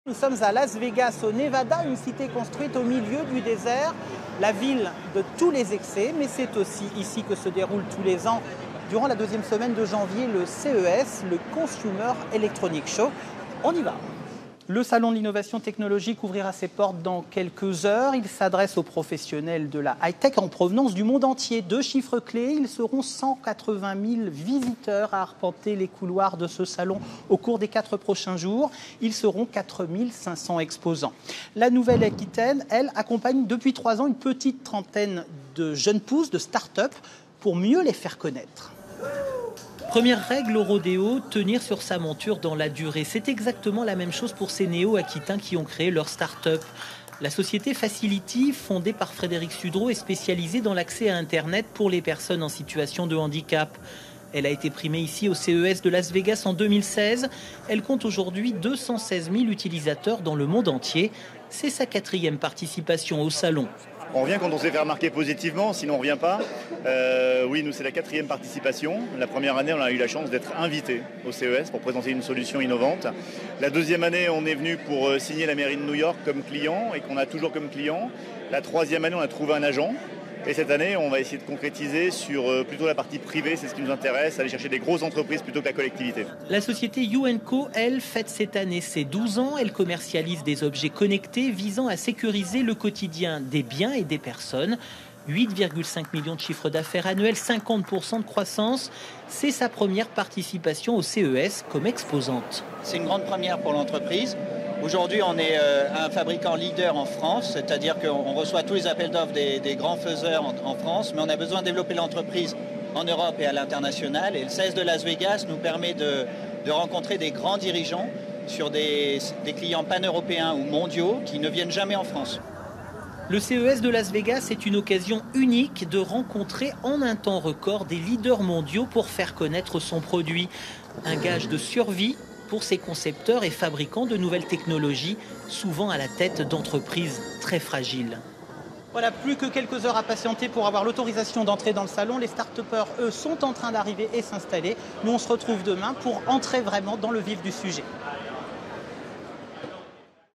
Les entreprises de Nouvelle-Aquitaine au CES de Las Vegas
28 entreprises de Nouvelle-Aquitaine dont 14 start-up participent à l'édition 2019 du CES de Las Vegas du 8 au 11 janvier.
Reportage de Frédéric Cartaud, Cédric Cottaz et Philippe Ritaine
Intervenants : Frédéric Sudraud, Fondateur de la start-up Facil'iti - Limoges (87) et Romuald Vetro, Président de Hu and Co - Bordeaux (33)